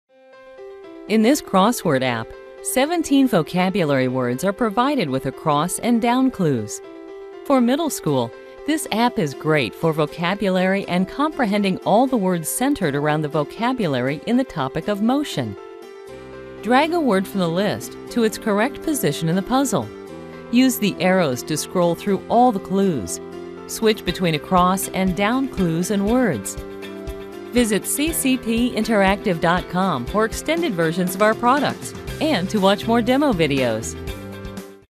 CC7554 Motion: Crossword Mini
In this crossword mini 17 vocabulary words are provided with across and down clues. This mini is great for vocabulary and comprehending all the words centered around the vocabulary in the Motion lesson plan. Vocabulary Words include: acceleration, deceleration, distance, frequency, friction, graph, mass, medium, motion, pitch, resistance, rotations, seismic, speed, velocity, wavelength. Our content meets the Common Core State Standards and are written to Bloom's Taxonomy.

for related minis and extended versions of all our products.